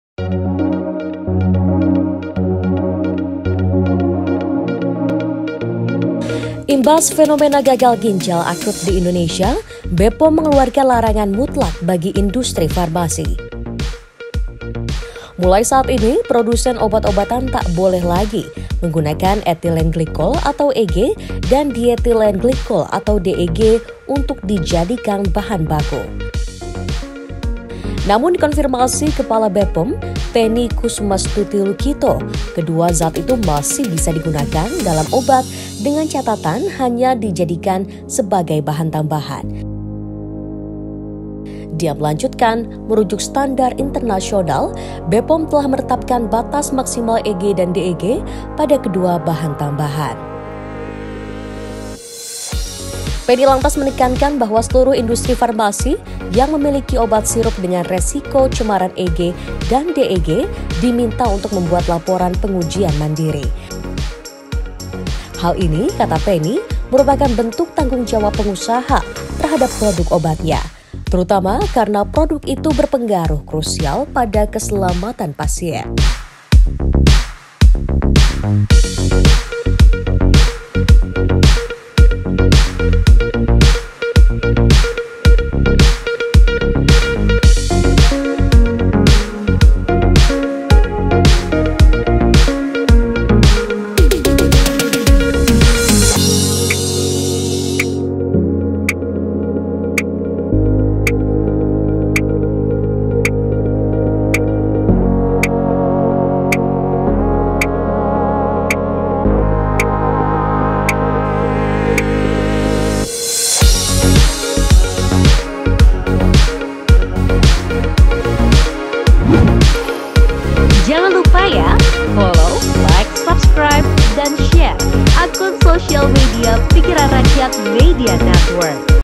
BPOM Haramkan Industri Farmasi Jadikan EG dan DEG sebagai Bahan Baku, Buntut Gagal Ginjal Akut
Imbas fenomena gagal ginjal akut di Indonesia, Badan Pengawas Obat dan Makanan (BPOM) mengeluarkan larangan mutlak bagi industri farmasi.
Mulai saat ini, produsen obat-obatan tak boleh lagi menggunakan etilen glikol (EG) dan dietilen glikol (DEG) untuk dijadikan bahan baku.
Namun, dikonfirmasi Kepala BPOM, Penny Kusumastuti Lukito, kedua zat itu masih bisa digunakan dalam obat, dengan catatan hanya dijadikan sebagai bahan tambahan.
Siti Aisah Nurhalida Musthafa/PRMN
Vid. Editor: Gesang/PRMN